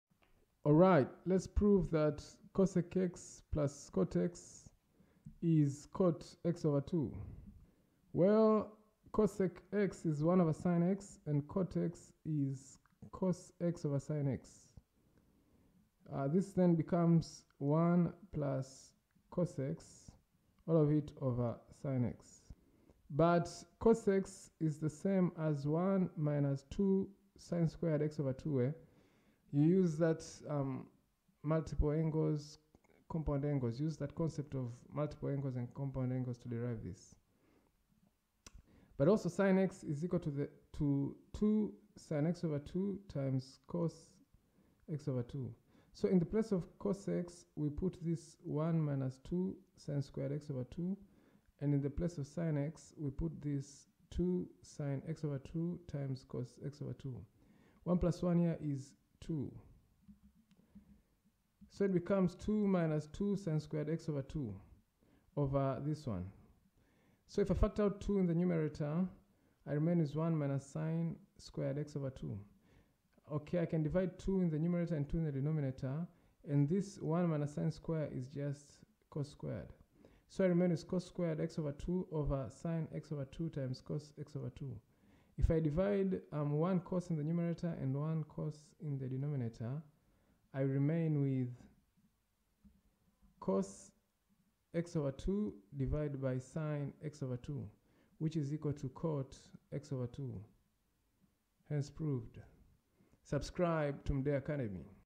Proof that cot(x) + cosec(x) =
Do you know that cot(x) + cosec(x) = cot(x/2) ? Lets prove it